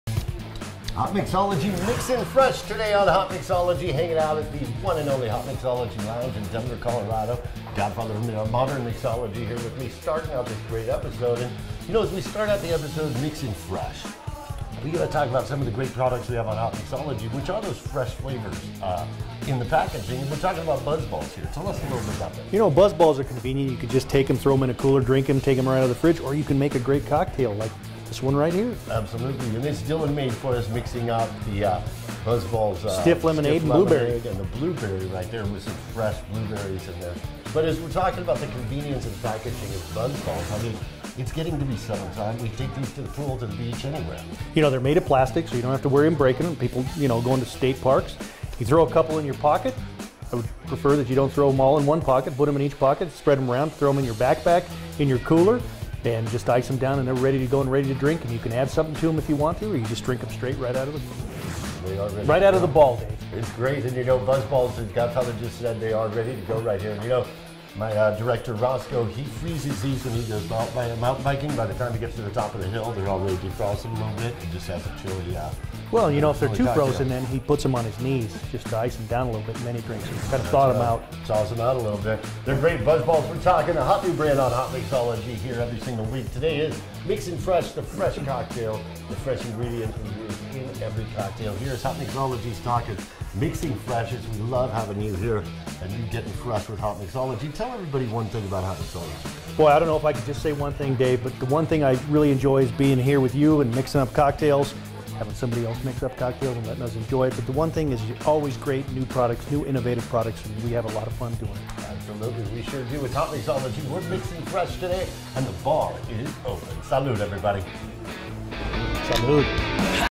BuzzBallz FRESH COCKTAIL ON HOTMIXOLOGY
BuzzBallz in a Fresh Cocktail as we hear all about the great BuzzBallz from the Godfather Of Modern Mixology only on Hotmixology